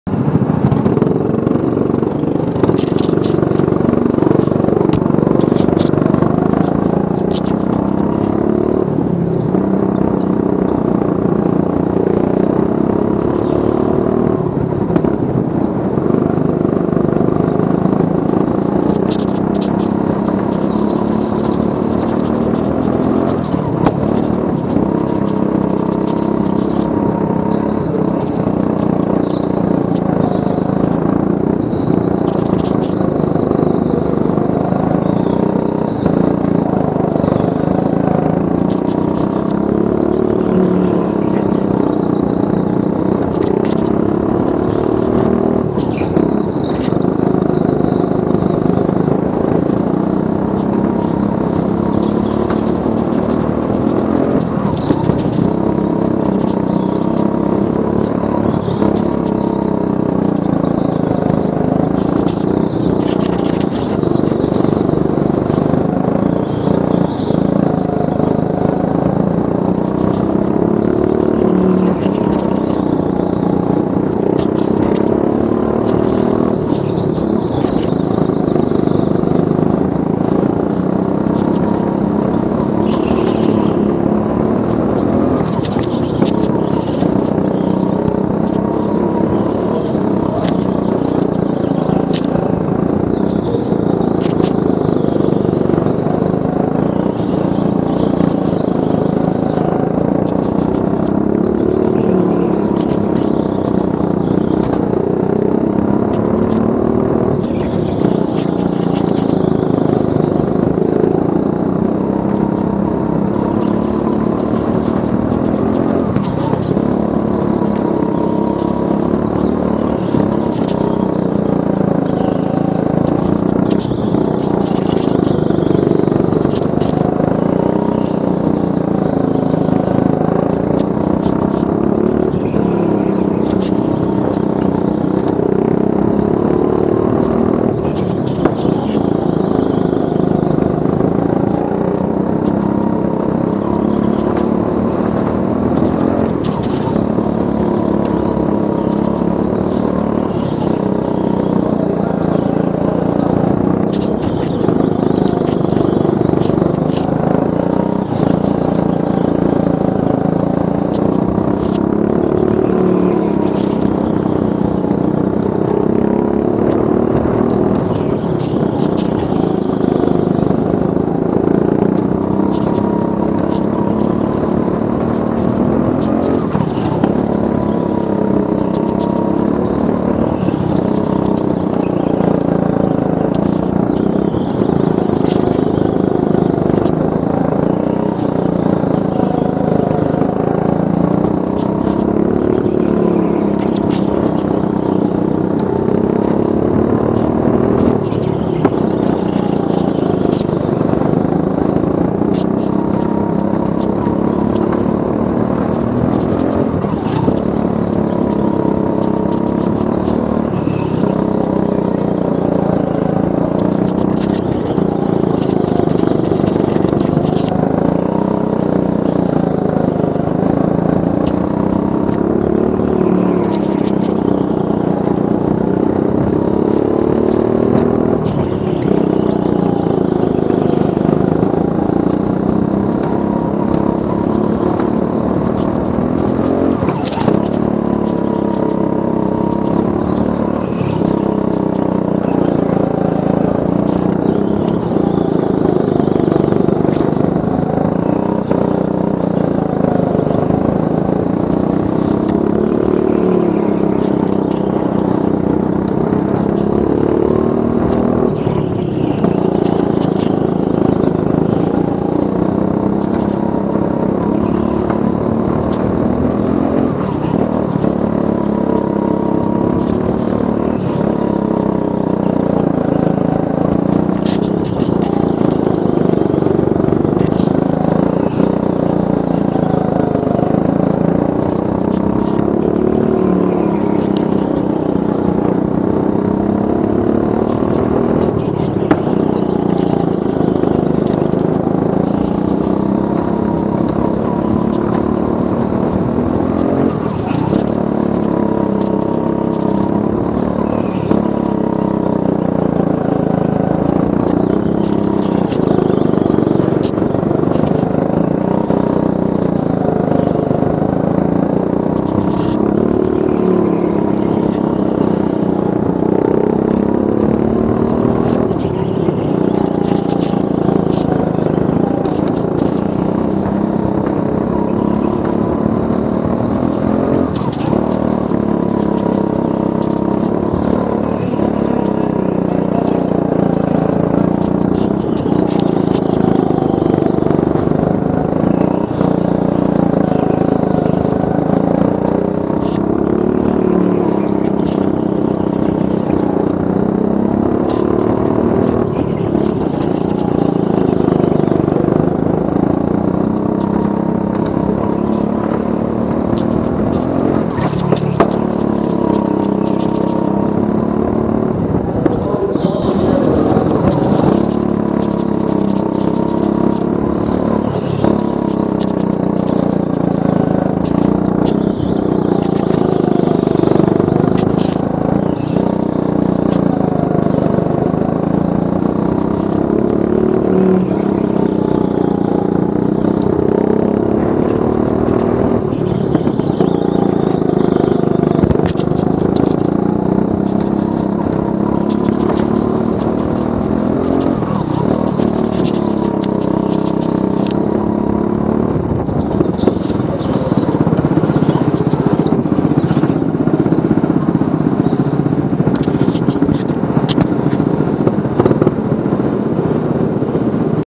Teamsport Gosport: Chase the Ace 1 lap Quali and 5min Practice (28/04/16)
Hey guys, this was our 5min practice session and 1 shot qualifying. I qualified 2nd.